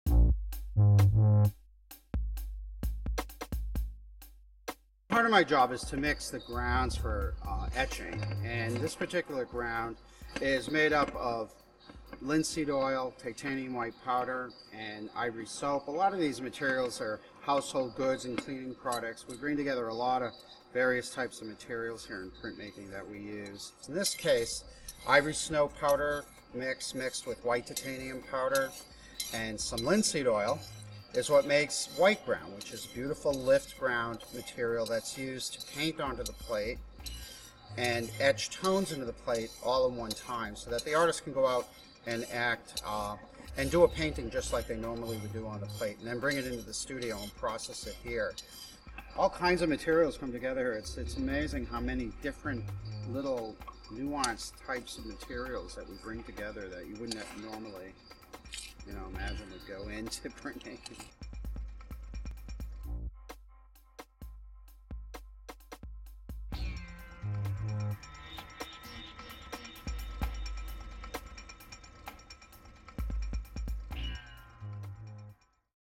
Alvin Oulette, Studio Manager, MassArt Printmaking Program
Documentary Editing workshop class project -- MassArt Summer Film School in Boston, June 2014.